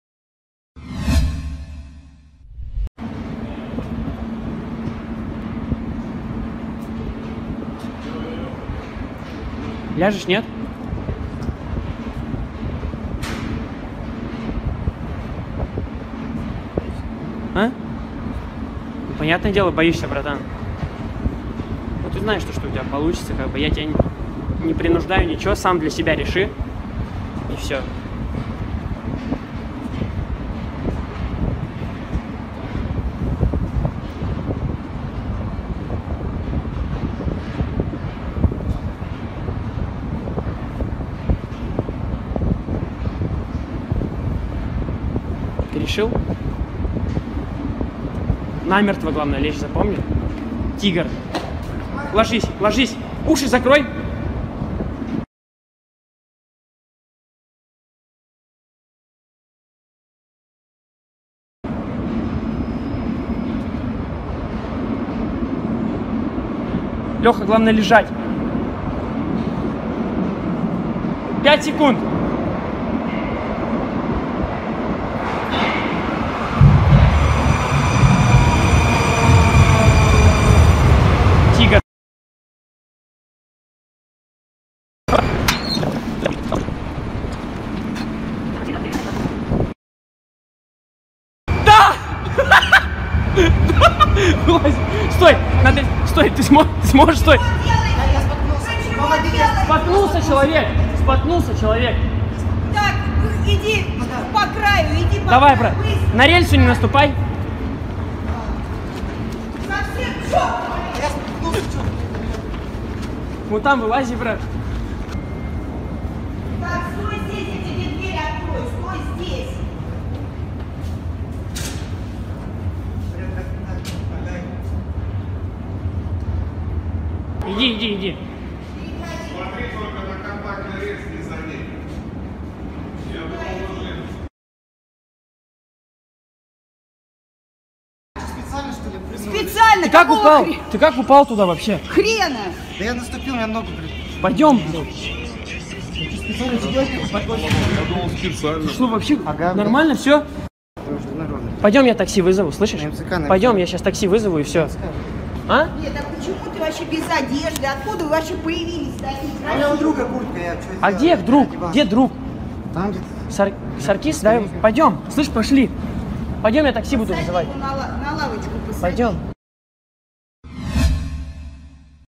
Saltó a las vías del Metro para ganar seguidores en las redes sociales
Un ruso loco

En Rusia hay algunos dispuestos a lo que sea con tal de ganar seguidores en las redes sociales, al menos esto es lo que deja en claro este vídeo en el que se ve como un joven se lanza a las vías del Metro instantes antes de que llegue un convoy. Su amigo grabó su imprudencia con su teléfono.